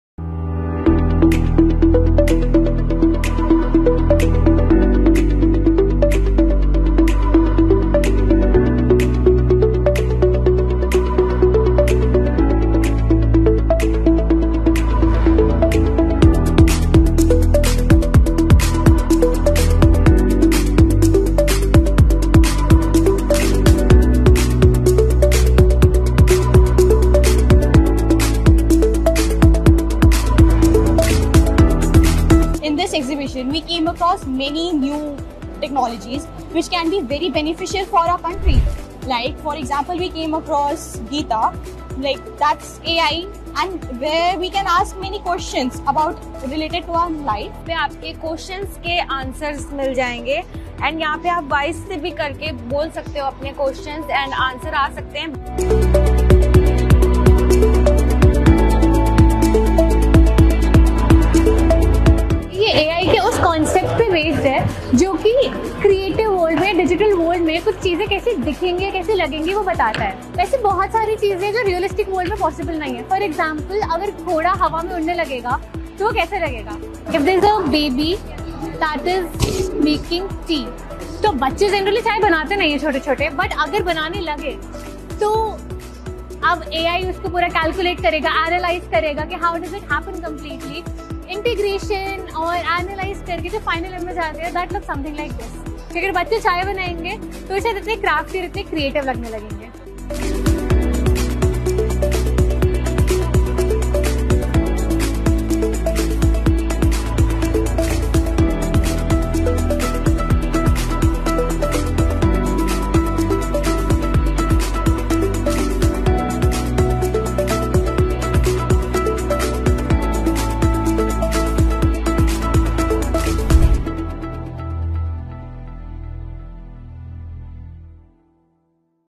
The Digital Experience Centre at the G20DEWG Meet in Lucknow
IndiaTechade dream would be realised on the strength of its innovators- PM

The Digital Experience Centre at the G20DEWG Meet in Lucknow showcased the limitless possibilities in AI for both innovators & users.